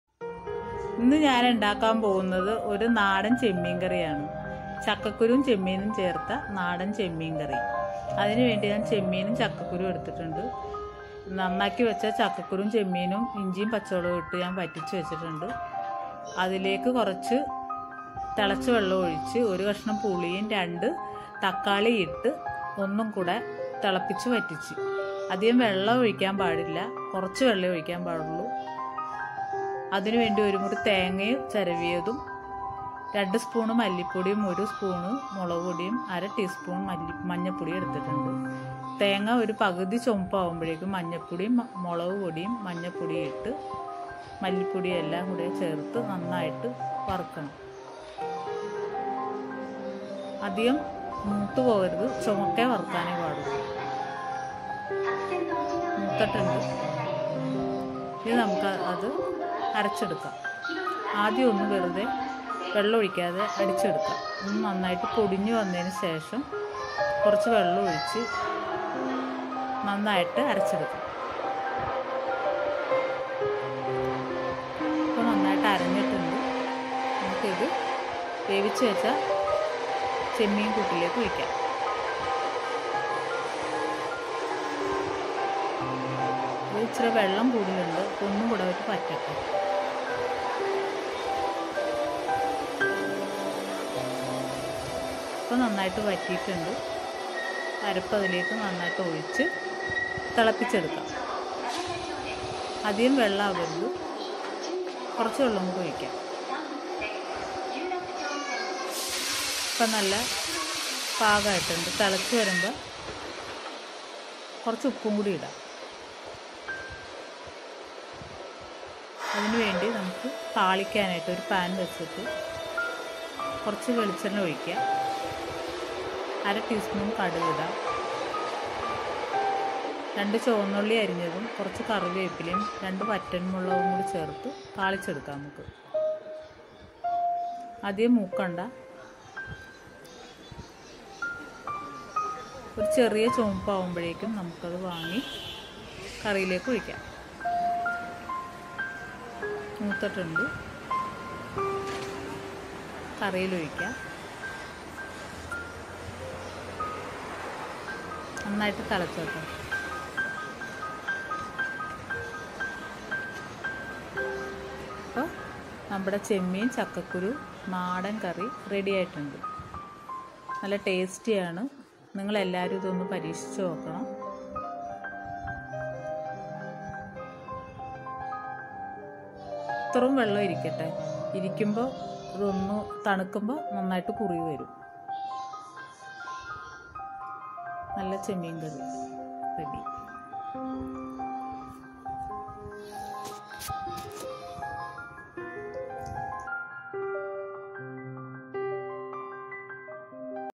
ചക്കക്കുരു ചെമ്മീൻ തീയൽ/Chemmeem theeyal/നാടൻ വറുത്തരച്ച കറി/prawns curry
ingredients:-

ചെമ്മീൻ(prawns)-250gm
ചക്കക്കുരു(Jack fruit seeds)-150gm
ginger -small piece
onion - 1nos
green chillies-3nos
curry leaves
coconut - half
turmeric powder-1/2 tspn
chilli powder- 2 1/2 tspn
salt
mustard
coconut oil